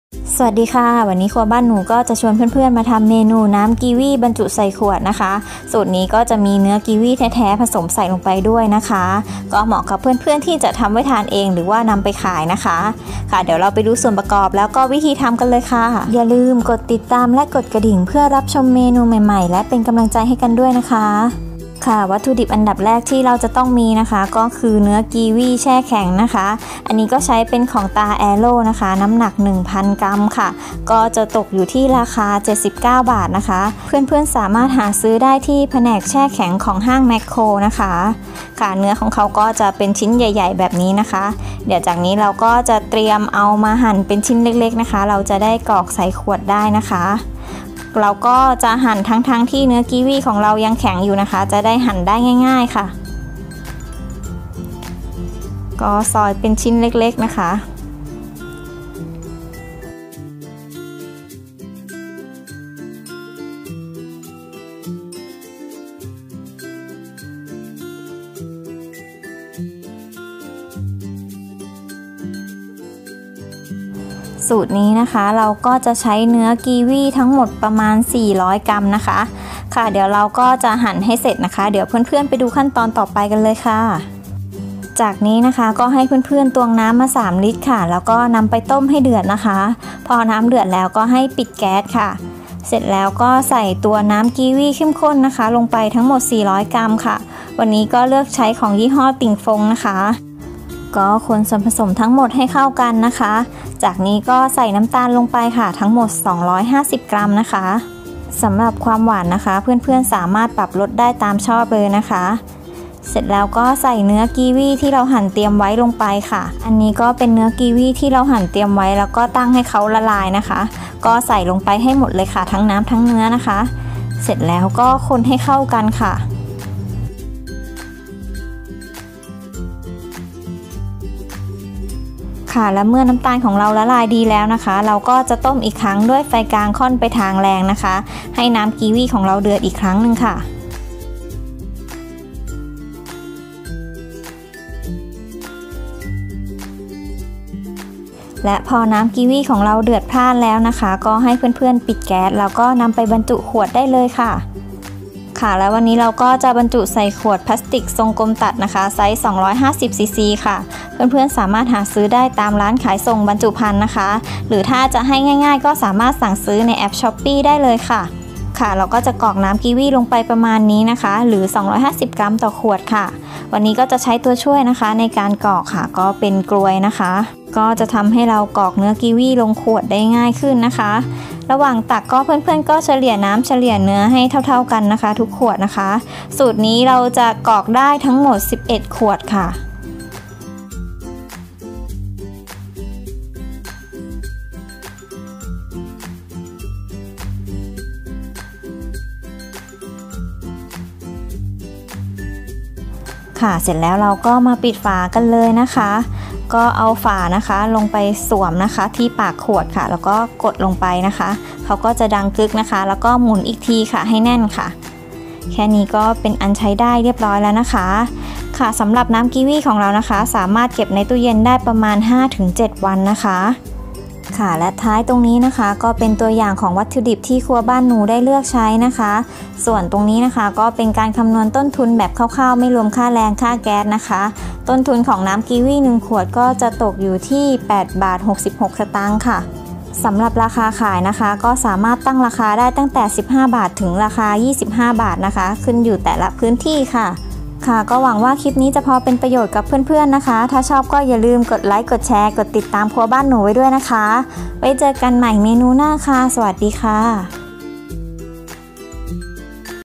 วิธีทำน้ำกีวี่บรรจุขวด น้ำผลไม้ เมนูเครื่องดื่ม ครัวบ้านหนู
วันนี้ครัวบ้านหนูก็จะชวนเพื่อนๆมาทำน้ำกีวี่ผสมเนื้อบรรจุใส่ขวดไว้ทานเอง หรือจะทำขายก็ได้ค่ะ

เนื้อกีวี่แช่แข็ง 400 กรัม
น้ำกีวี่สควช 400 กรัม
น้ำร้อน 3 ลิตร

ตัวอย่างวัตถุดิบและเครื่องมือที่ใช้
ขวดทรงกลมตัด 250 cc 120 ใบ ราคา 235 บาท

เครื่องชั่งดิจิตอล สูงสุด 10 กิโลกรัม ราคา 52 บาท

เครื่องชั่งดิจิตอล 1 ตำแหน่ง ราคา 89 บาท

Ingredients
400 g Frozen kiwi
400 g Kiwi concentrated
3 lirte Hot water
250 g Sugar